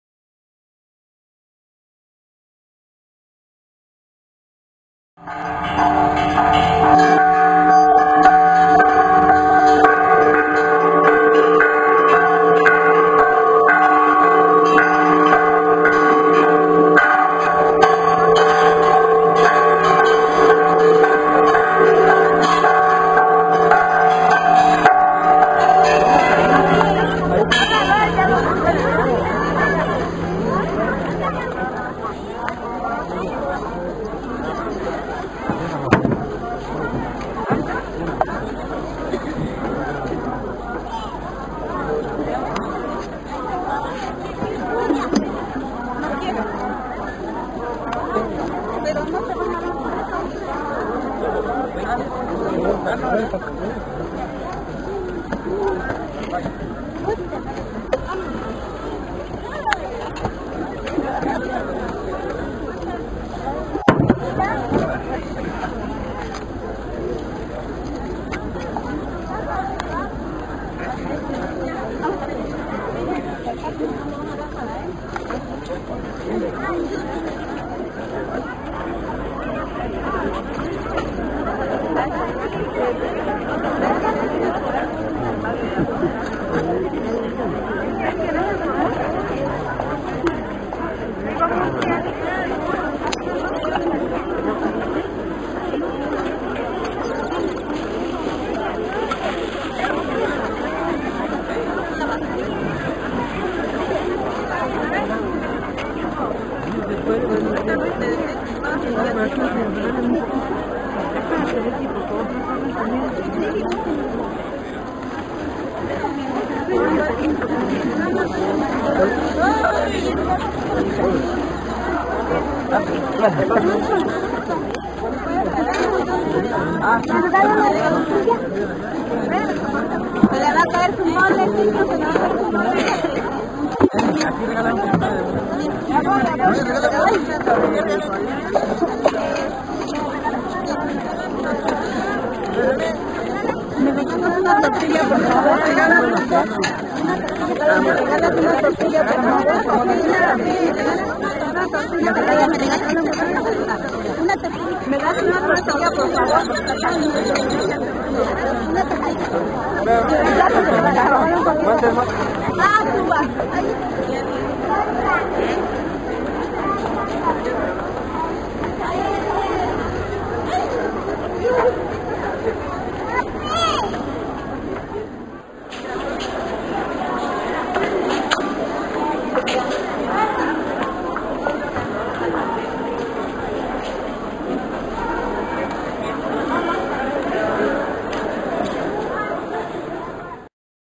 Semana Santa en Oaxaca 2011 (Jueves Santo - Repartición del Coloradito y Atole)
LEER NOTA COMPLETA Como desde hace muchos años, en la comunidad de San Francisco Telixtlahuaca se vivió una de las tradiciones mas bonitas de estos tiempos con motivo de la celebración del jueves santo: La Bendición del Pan y Repartición del Coloradito y Atole.